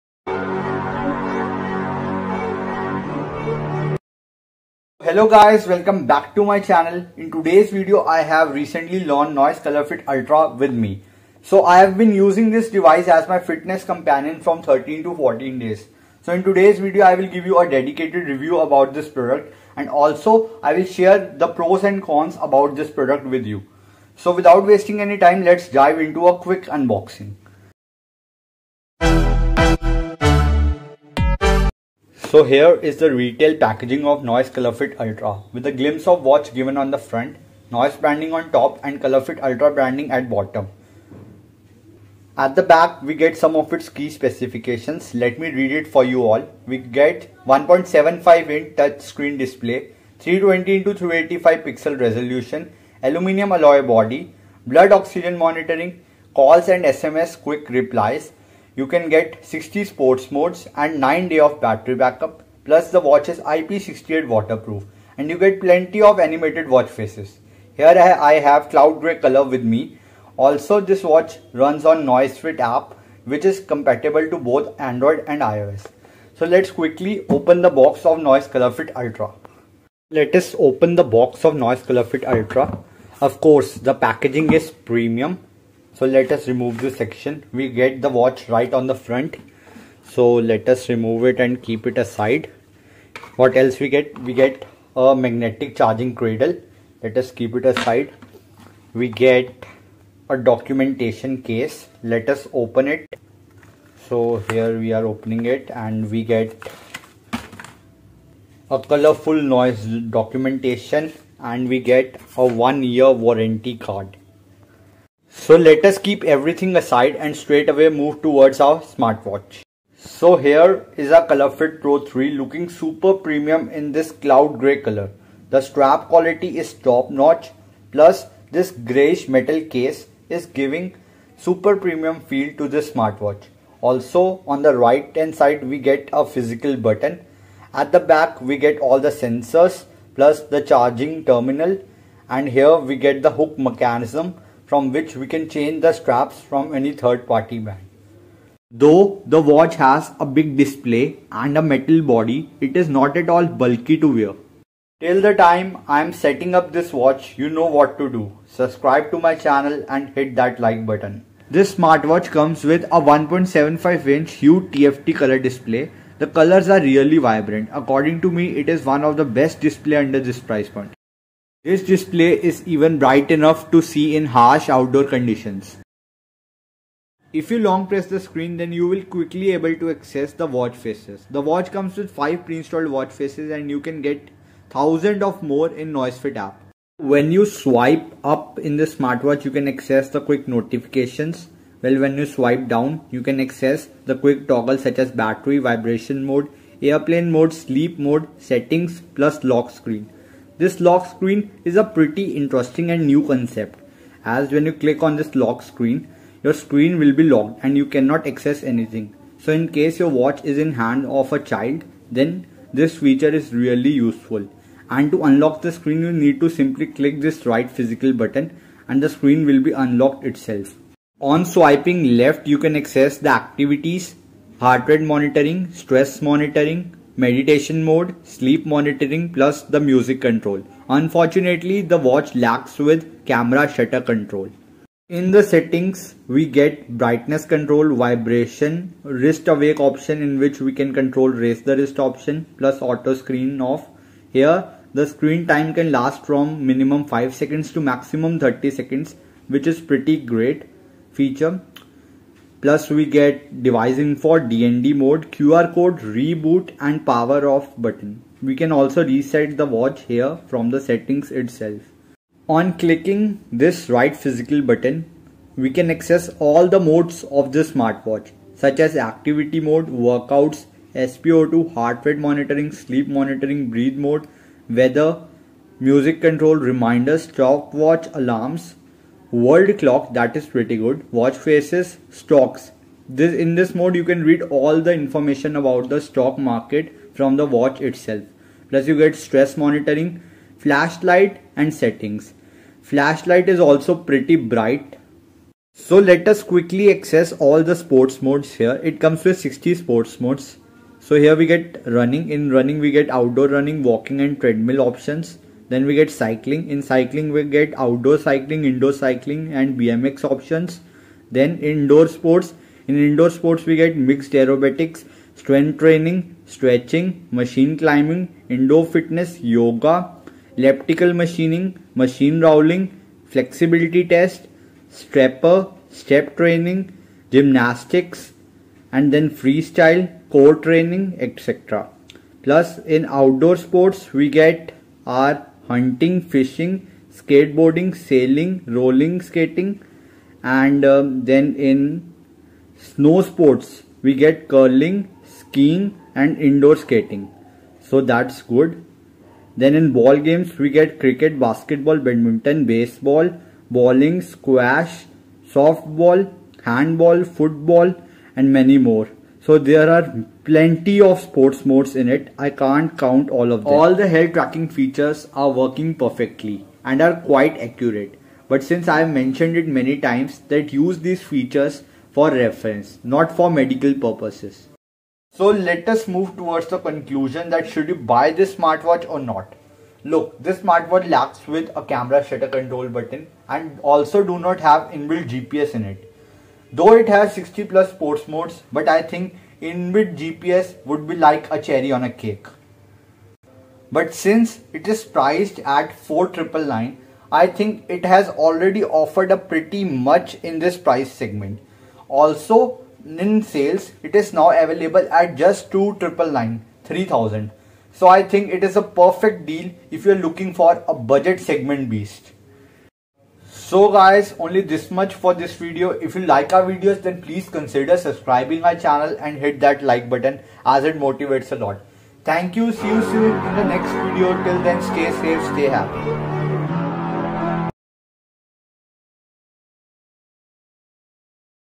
Noise Colorfit Ultra Unboxing and Review | Prabhnoor Singh
This video contains the unboxing and review of Noise Colorfit Ultra. All the features ar discussed in this video to make it easy for you to make buying decision.

Features:
•Heart Rate Monitoring
•SPO2
•Stress Monitoring
•60+ Sports Mode
•Meditation Mode
•Alarms
•Call and Messages Alert
•Music Control
•Compass
•Stock Update
•Flashlight

Price:4999/-
Price at Amazon great Indian Sale: 3299/-